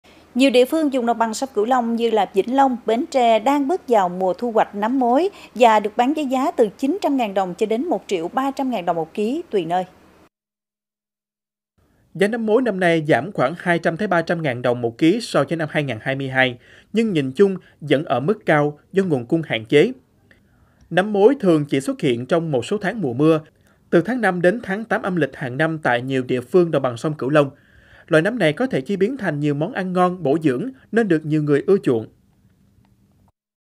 Giá nấm mối giảm nhưng vẫn ở mức cao| Cần Thơ TV
Nhiều địa phương vùng ÐBSCL như Vĩnh Long, Bến Tre đang bước vào mùa thu hoạch nấm mối và được bán với giá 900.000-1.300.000 đồng/kg, tùy nơi.